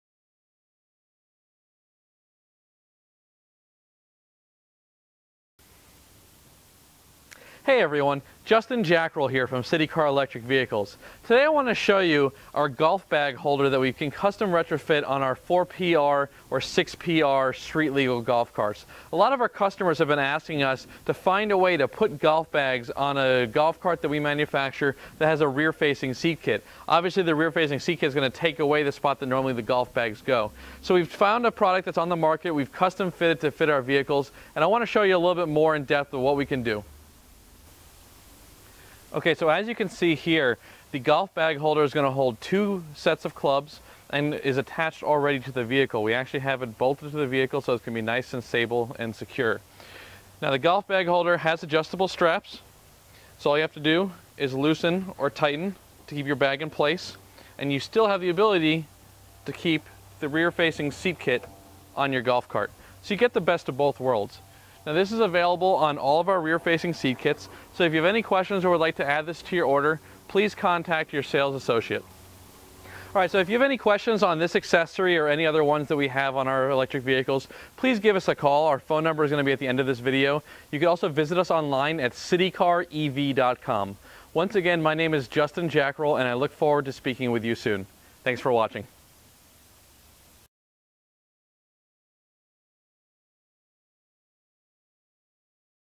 Golf Bag Holder for Golf Carts with Rear Facing Seat, by Bintelli Electric Vehicles
This video shows a custom kit we installed on a golf cart with a rear facing seat kit. This addition allows for two golf bags to be carried without disturbing the rear passengers. All of our street legal golf carts, electric shuttles, low speed vehicles, and standard golf carts are built in the USA.